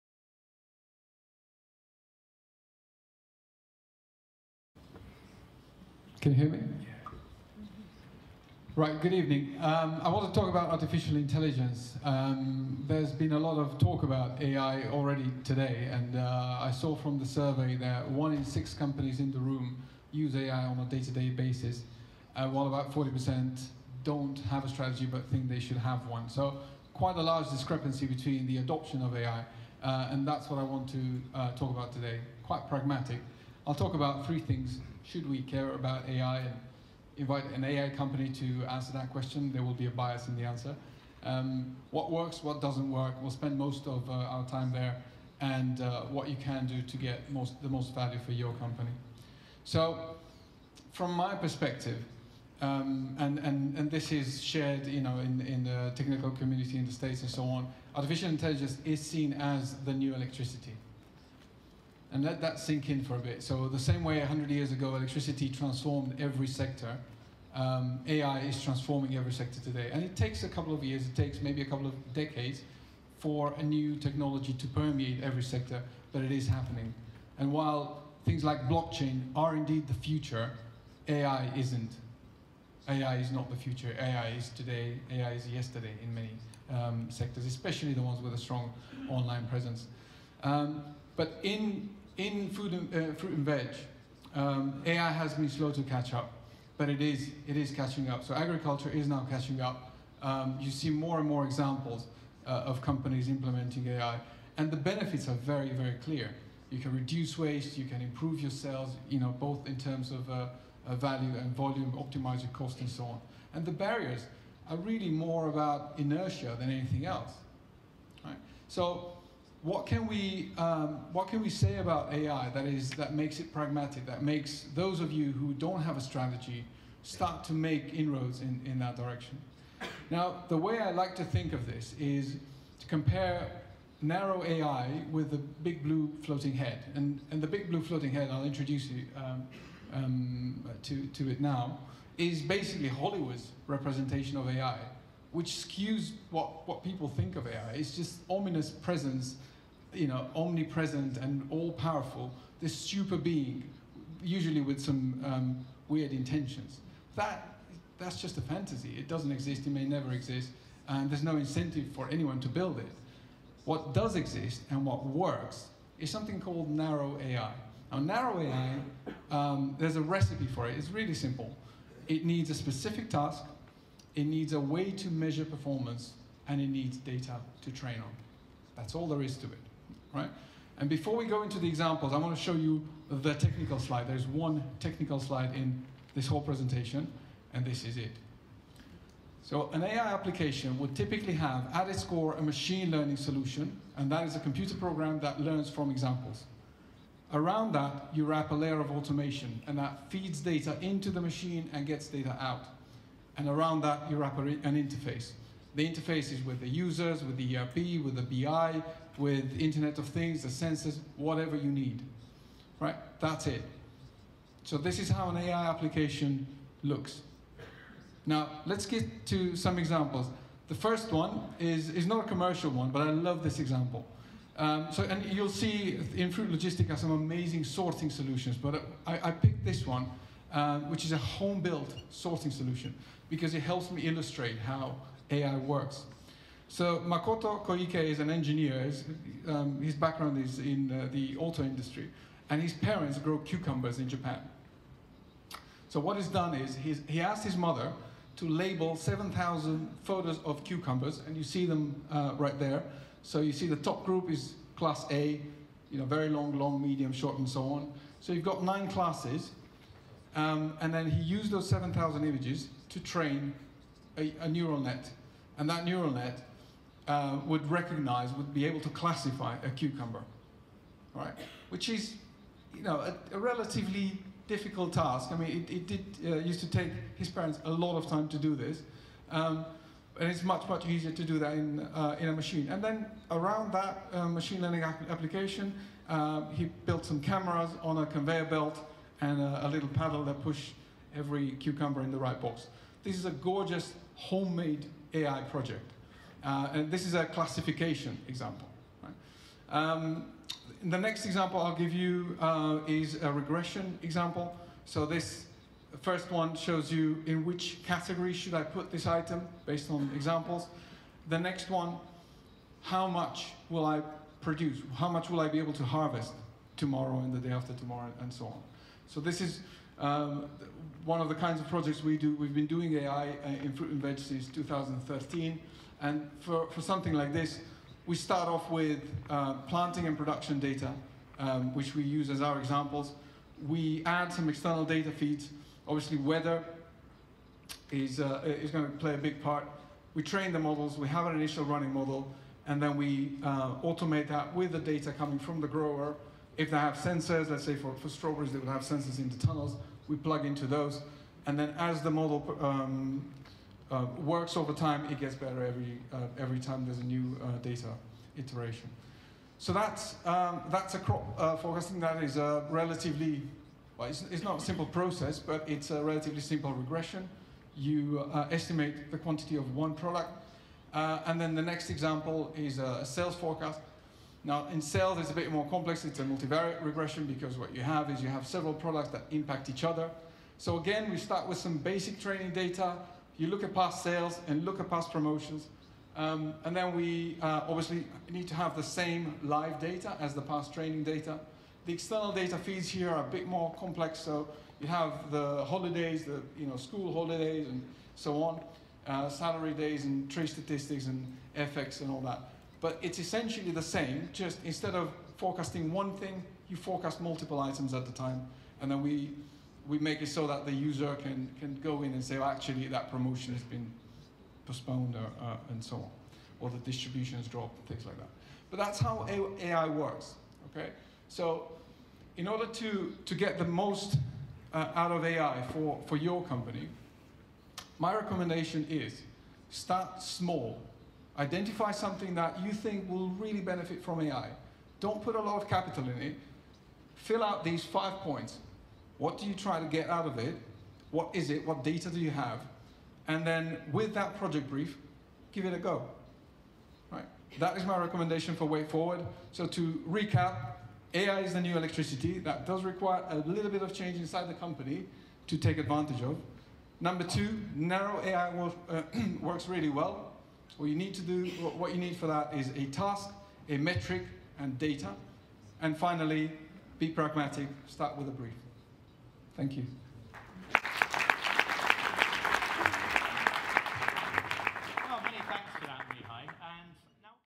Applied Artificial Intelligence in Fruit and Veg
A brief look at some ways Artificial Intelligence solutions are already transforming fruit and veg, presented by Mihai Ciobanu of Fresh4cast at the Fruitnet World of Ideas event at Fruit Logistica 2018.